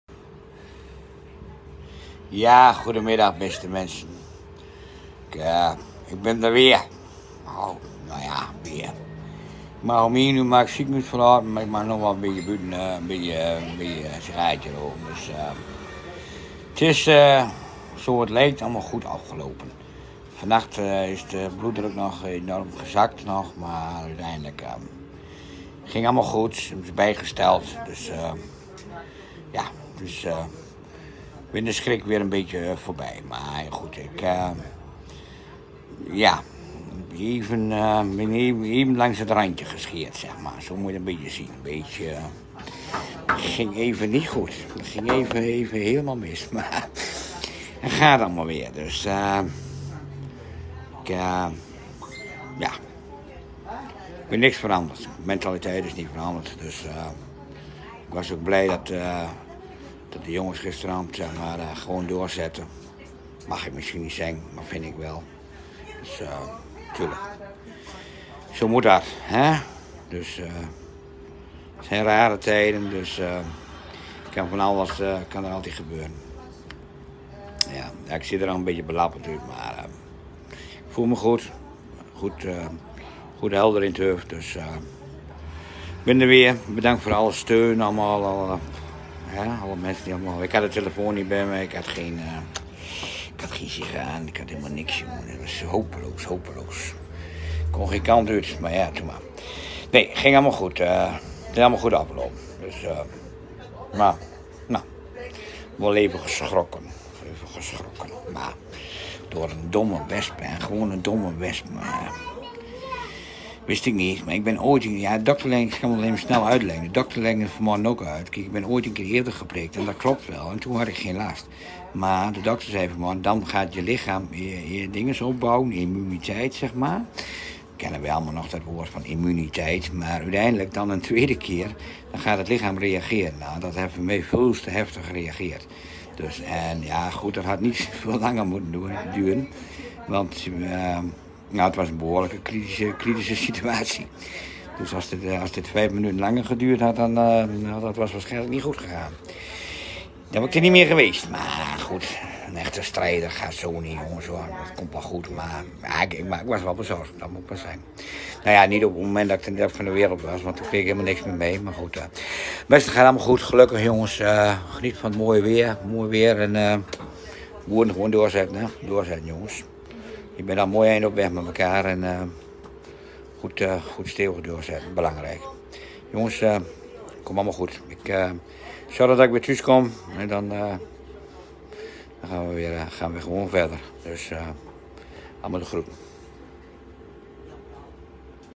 Opruier Jan Huzen zuigt weer aan een sigaar!
Nou nou nou zeg, dat was me wat gisteravond, opruier Jan Huzen ging neer! En hij had het nog net zo naar zijn zin tussen de terrorboeren, vervelend!

Natuurlijk werd er weer volop gespeculeerd maar wappie Jantje was gewoon geprikt door een wesp. En ja, dat kan vervelend uitpakken, heel naar ook hoor. Goh nou, wat een toestand.

Maar wappie Jan Huzen zit gewoon weer aan een smerige sigaar te zuigen en is oké.

Kan die fijn weer mee gaan lopen opruien met de terrorboeren.